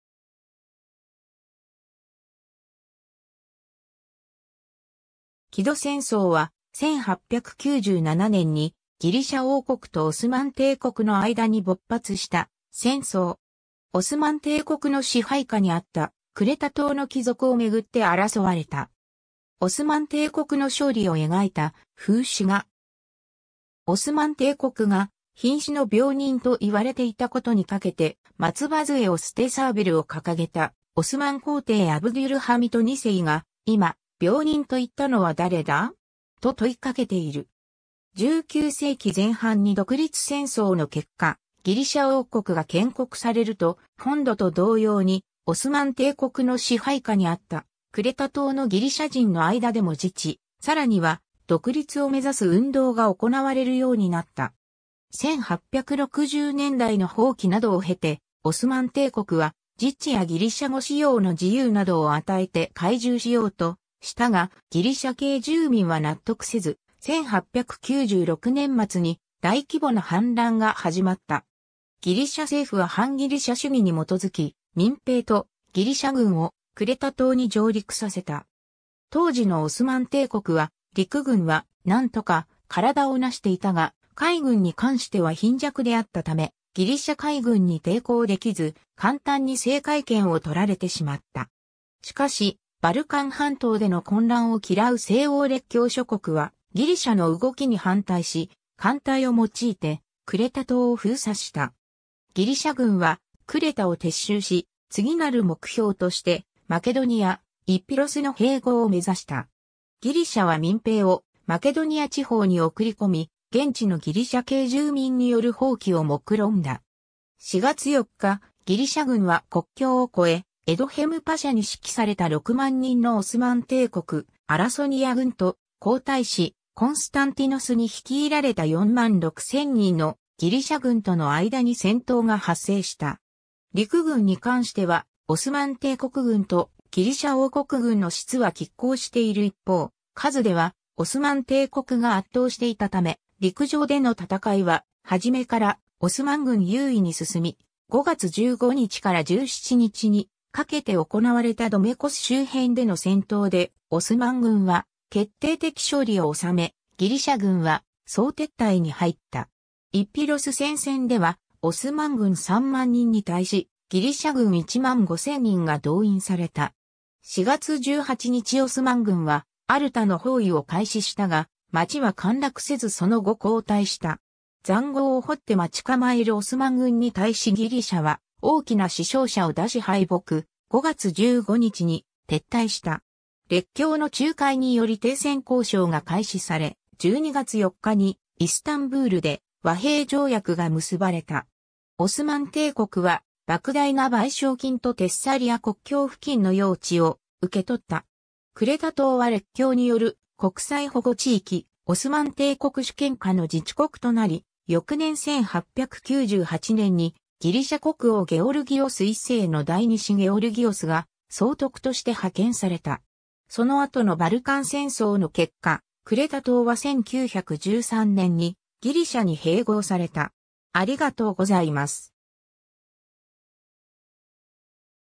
希土戦争 (1897年)
希土戦争（きとせんそう、ギリシア語: Ελληνοτουρκικός πόλεμος του 1897）は、1897年にギリシャ王国とオスマン帝国の間に勃発した戦争。
オスマン帝国の支配下にあったクレタ島の帰属を巡って争われた。
 オスマン帝国の勝利を描いた風刺画。
オスマン帝国が「瀕死の病人」と言われていたことに掛けて、松葉杖を捨てサーベルを掲げたオスマン皇帝アブデュルハミト2世が「今、病人と言ったのは誰だ?」と問い掛けている。
 19世紀前半に独立戦争の結果、ギリシャ王国が建国されると、本土と同様にオスマン帝国の支配下にあったクレタ島のギリシャ人の間でも自治、更には独立を目指す運動が行われるようになった。
1860年代の蜂起などを経てオスマン帝国は自治やギリシャ語使用の自由などを与えて懐柔しようとしたが、ギリシャ系住民は納得せず、1896年末に大規模な反乱がはじまった。
 ギリシャ政府は汎ギリシャ主義(メガリ・イデア)に基づき、民兵とギリシャ軍をクレタ島に上陸させた。
当時のオスマン帝国は陸軍はなんとか体を成していたが、海軍に関しては貧弱であったためギリシャ海軍に抵抗できず、簡単に制海権を取られてしまった。
しかしバルカン半島での混乱を嫌う西欧列強諸国はギリシャの動きに反対し、艦隊を用いてクレタ島を封鎖した。
 ギリシャ軍はクレタを撤収し、次なる目標としてマケドニア、イピロスの併合を目指した。
ギリシャは民兵をマケドニア地方に送り込み、現地のギリシャ系住民による蜂起を目論んだ。
4月4日ギリシャ軍は国境を越え、エドヘム・パシャに指揮された60,000人のオスマン帝国アラソニヤ軍と皇太子コンスタンティノスに率いられた46,000人のギリシャ軍との間に戦闘が発生した。
陸軍に関してはオスマン帝国軍とギリシャ王国軍の質は拮抗している一方、数ではオスマン帝国が圧倒していたため、陸上での戦いは始めからオスマン軍優位に進み、5月15日から17日にかけて行われたドメコス (ドョメケ)周辺での戦闘でオスマン軍は決定的勝利をおさめ、ギリシャ軍は総撤退に入った。
 イピロス戦線ではオスマン軍30,000人に対し、ギリシャ軍15,000人が動員された。
4月18日オスマン軍はアルタの包囲を開始したが、町は陥落せずその後後退した。
塹壕を掘って待ち構えるオスマン軍に対しギリシャは大きな死傷者を出し敗北、5月15日に撤退した。
 列強の仲介により停戦交渉が開始され、12月4日にイスタンブールで和平条約が結ばれた。
オスマン帝国は莫大な賠償金とテッサリア国境付近の要地を受け取った。
クレタ島は列強による国際保護地域、オスマン帝国主権下の自治国となり、翌年1898年にギリシャ国王ゲオルギオス1世の第二子ゲオルギオスが総督として派遣された。
その後のバルカン戦争の結果、クレタ島は1913年にギリシャに併合された。